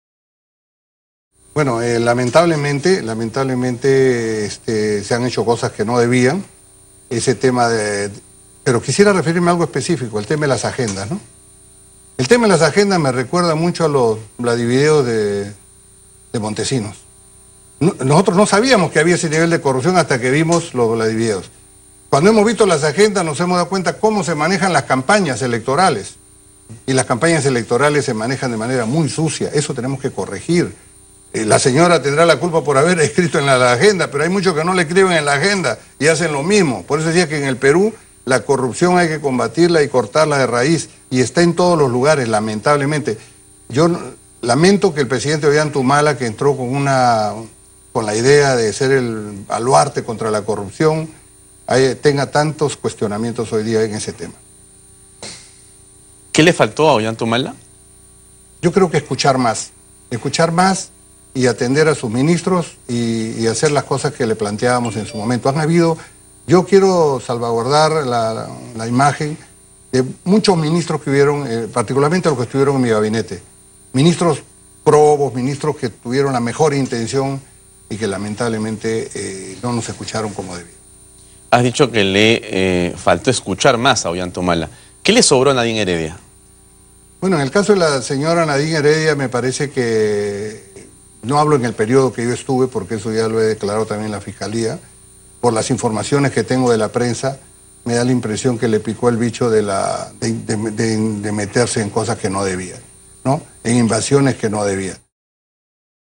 Óscar Valdés: "Las agendas me recuerdan a los vladivideos"
Para Óscar Valdés, el tema de la agendas es muy parecido al caso de los vladivideos. Valdés indicó que al expresidente Ollanta Humala le faltó oír más a sus ministros y que Nadine Heredia se metió donde no debía.